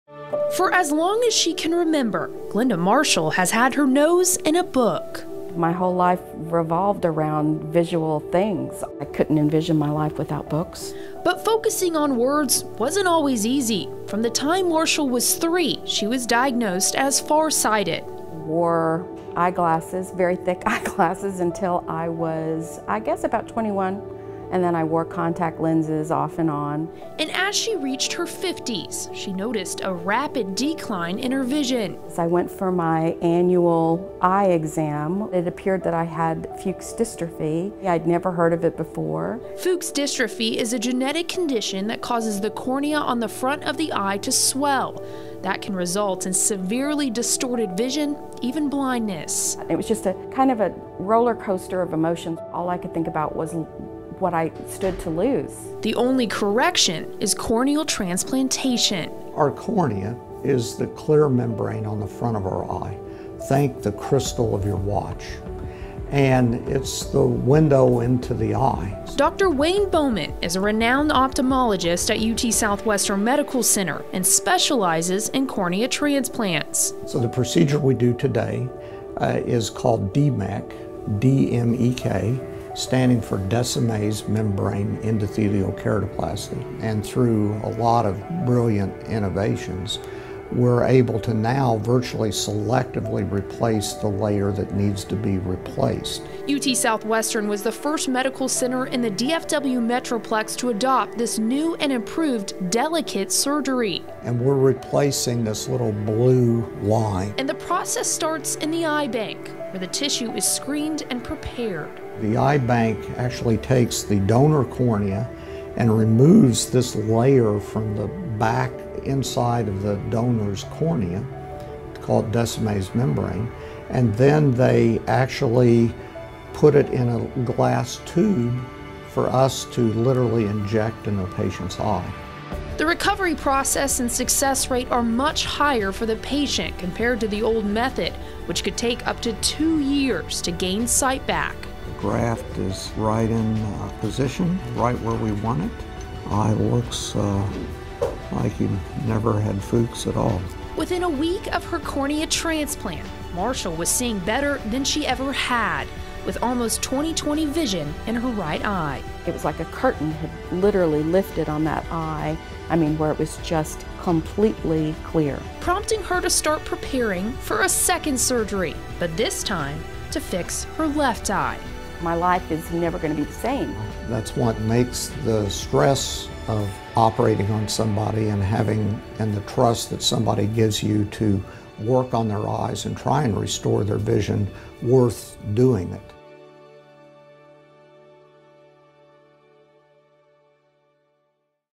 Normal vision in sight for cornea transplant patient
Glenda Marshall struggled for decades with thick glasses and foggy vision. Now following ​corneal transplantation she received at UT Southwestern Medical Center, she’s seeing better than ever.